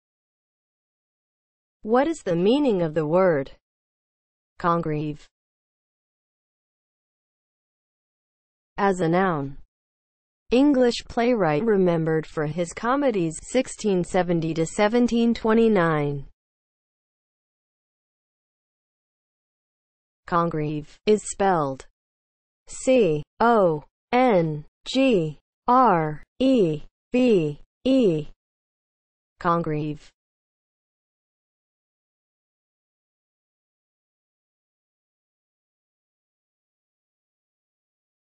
What is the meaning of the word CONGREVE?
Definition and spelling of the word CONGREVE.

(00:00) Intro
(00:07) As a noun
(00:18) Spelling

As a noun (00:07):
• English playwright remembered for his comedies (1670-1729)